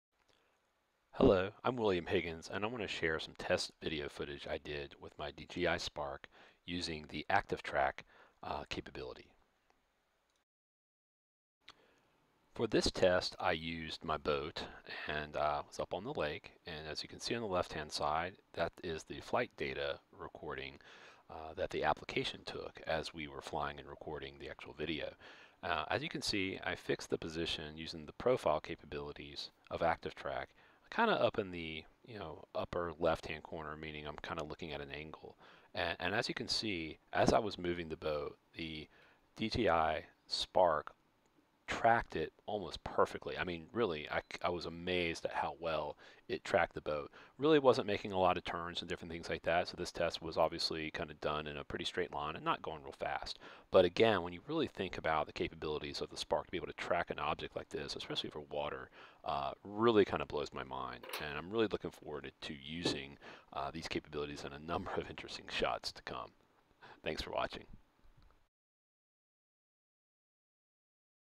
DJI Spark ActiveTrack Test - Tracking My Boat
This is a short test video of the ActiveTrack capability using my new DJI Spark.  This test demonstrates how the DJI Spark can track my moving boat for about a minute.